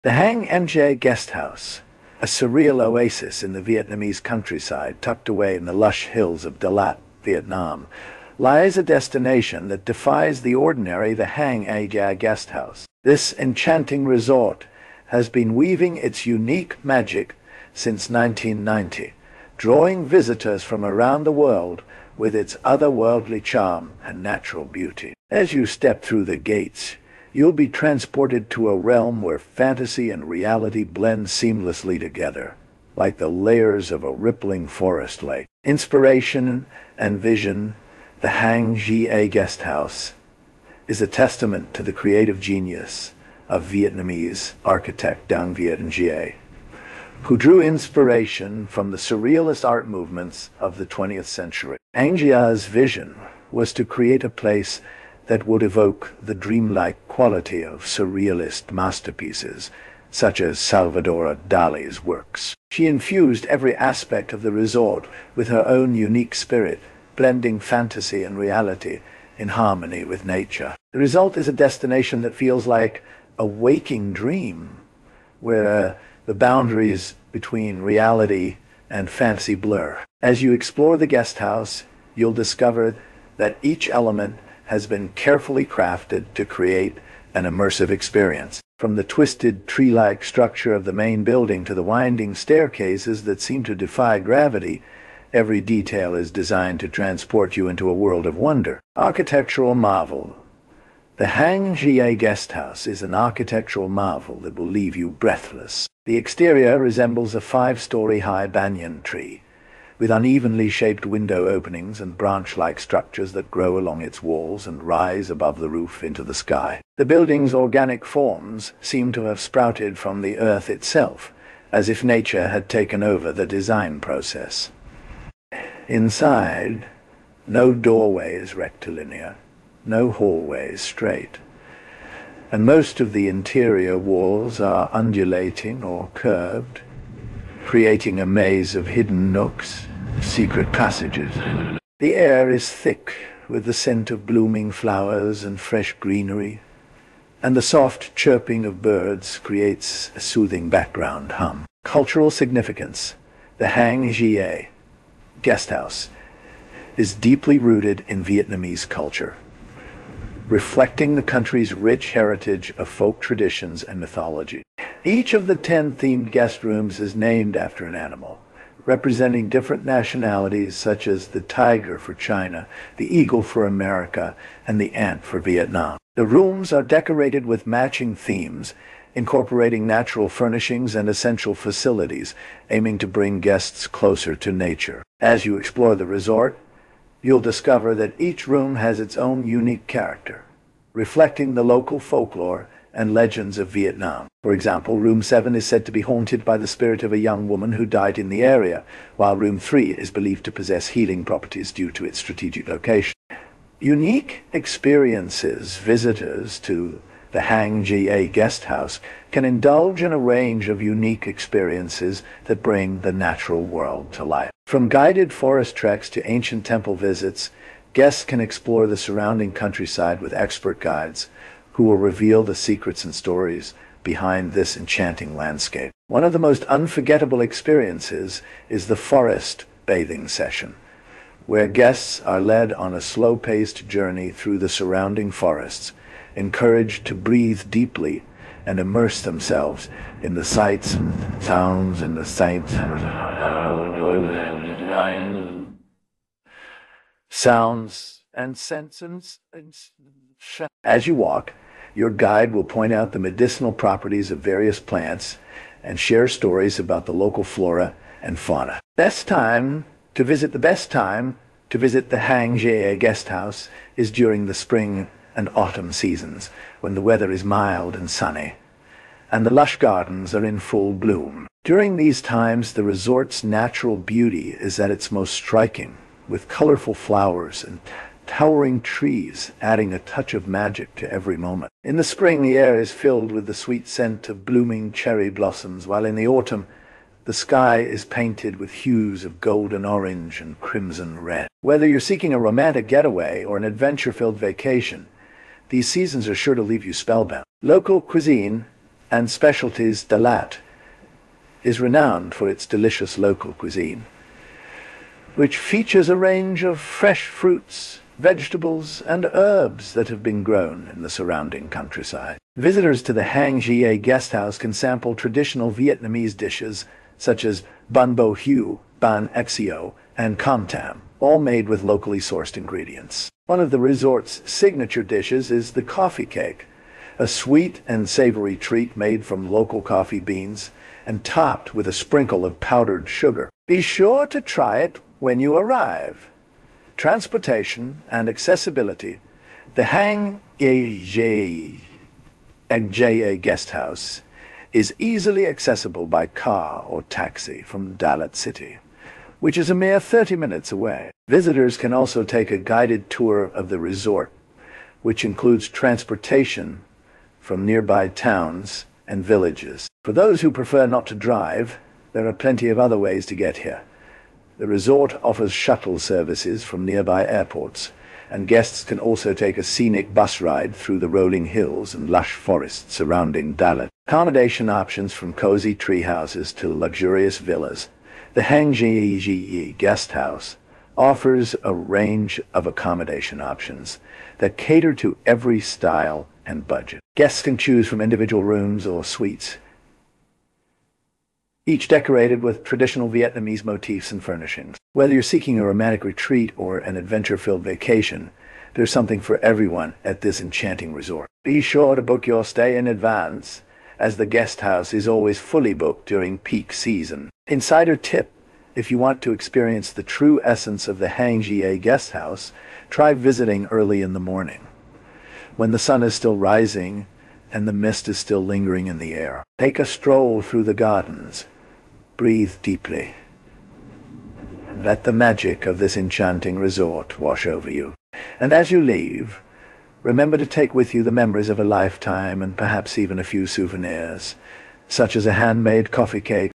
**The Enchanting Hang Nga Guesthouse: Uncovering the Mystique of Vietnam's "Mystic House"**
Step into a world of fantasy and adventure in Hanoi, Vietnam, at the mesmerizing Hang Nga Guesthouse, also known as the "Mystic House". This 19th-century architectural wonder is a must-visit destination for anyone fascinated by history, culture, and the unusual. In this video, we'll take you on a journey through time to explore the secrets of this enigmatic guesthouse, which boasts intricately carved wooden dragons, twisting staircases, and labyrinthine corridors that will leave you spellbound.

Join us as we delve into the rich historical background of Hanoi, learn about the cultural significance of the Hang Nga Guesthouse, and discover its most impressive attractions, including the stunning architecture, beautiful gardens, and unique artifacts on display. We'll also share with you some insider tips for making the most of your visit, from navigating the best times to explore to trying local specialties that will tantalize your taste buds.

Whether you're a history buff, a culture vulture, or simply looking for an unforgettable experience in Vietnam, this video is for you! So sit back, relax, and get ready to be transported to a world of wonder and magic at the Hang Nga Guesthouse.